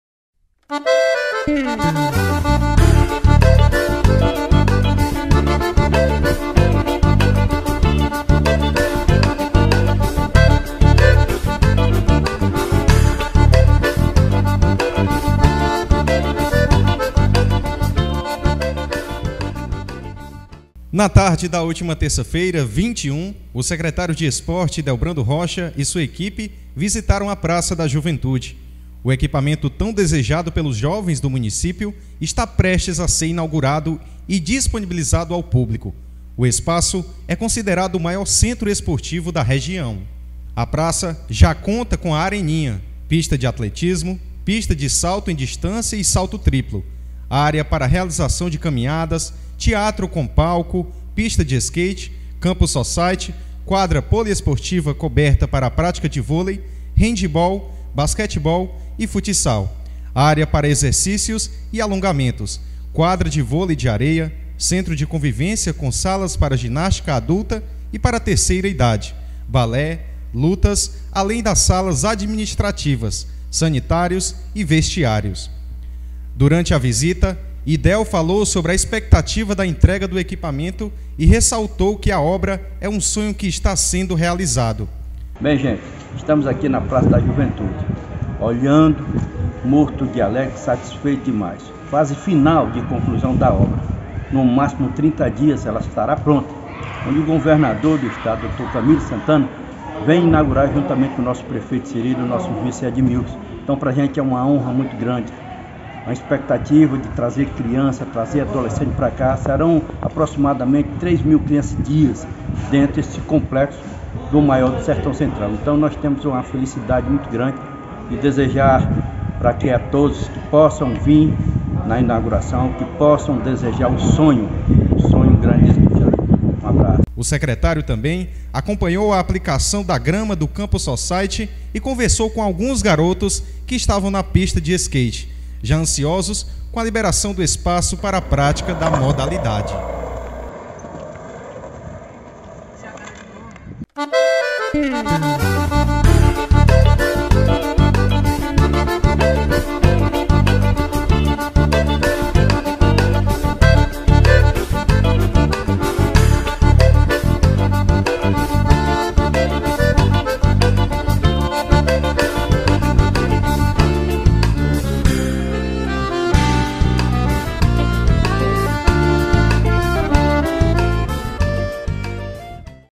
Secretário de esportes visita andamento de obras da praça da juventude em Quixeramobim
Envie seu vídeo para nossa equipe pelo WhatsApp, (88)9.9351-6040